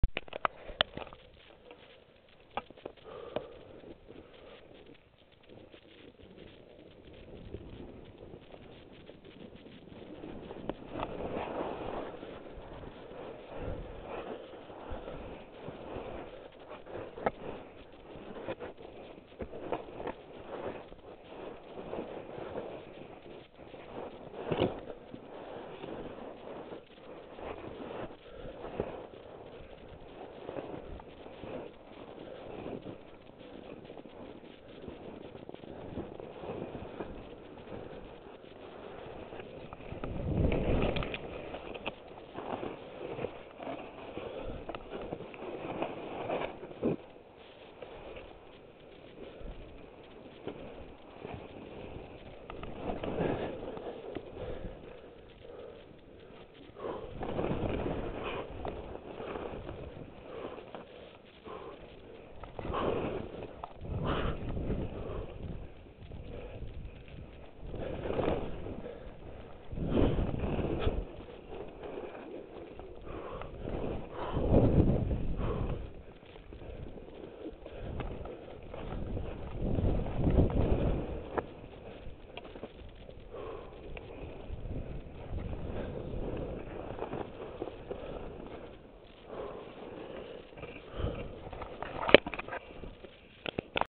MJ Chute
First time down... lots of rocks at the top but still lots of fun! (go pro was steamed up)